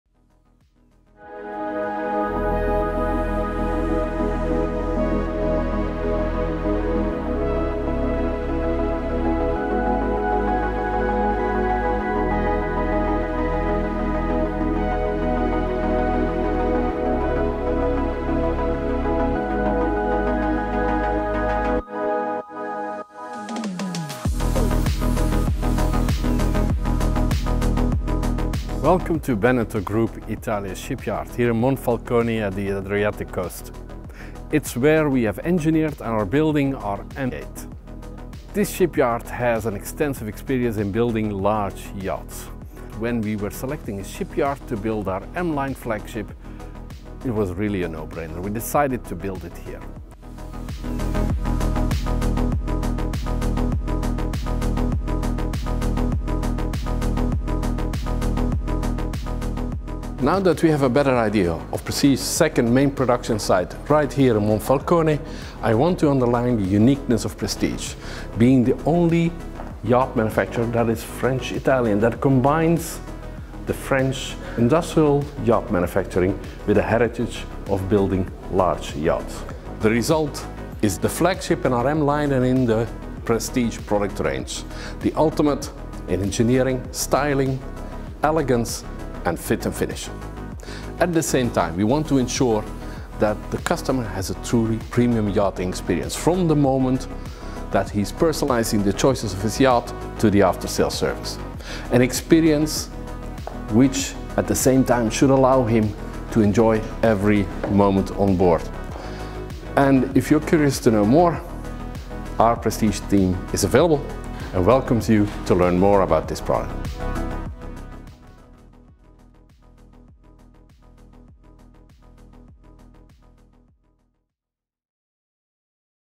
M8: Behind the scenes at the production facility
Art de Vivre in Harmony
Discover new horizons and reconnect nature, with yourself, and with others in a spacious, refined, luxurious environment:  this is the Art de Vivre offered by the new M8.